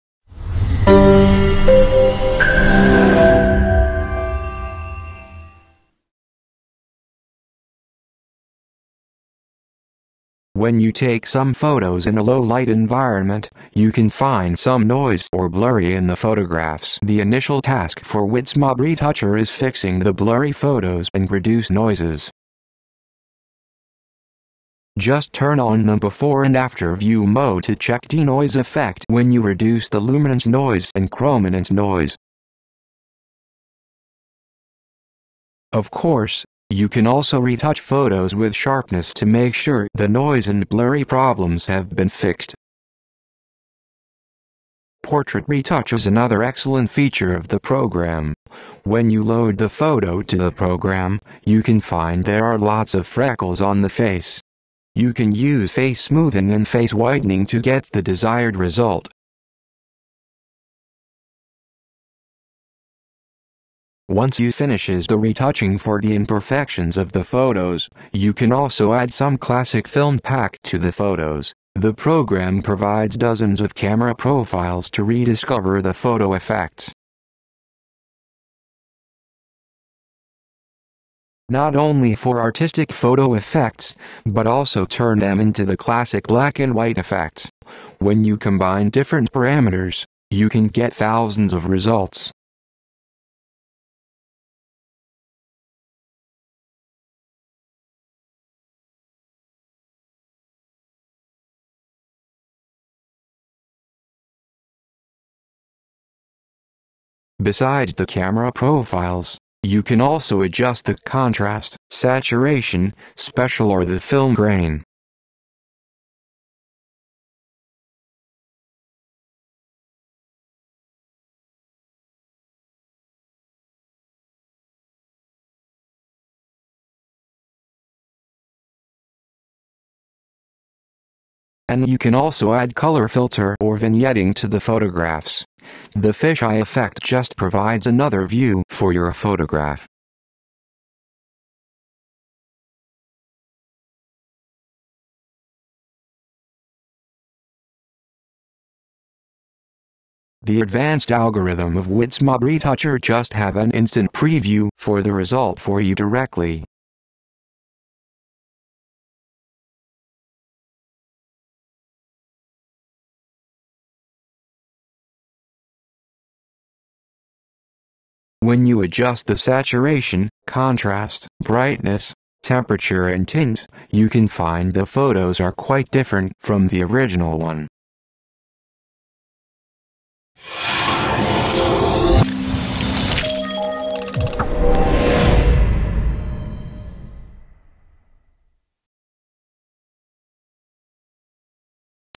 WidsMob Retoucher Video Tutorial - How to Use WidsMob Retouching to Retouching Your Photos
This video tutorial mainly tells you how to use WidsMob Retoucher photo editing features including photo denoise, portrait retouch, film pack, fish eye view and adjustments.

1. Adjust photo effects with basic photo editing tools.
2. Retouch portraits to get smooth and white skin.
3. Reduce noise to fix blurry photos.
4. Access lomo effect without using a Lomography Camera.
5. Analog film camera effects with ease.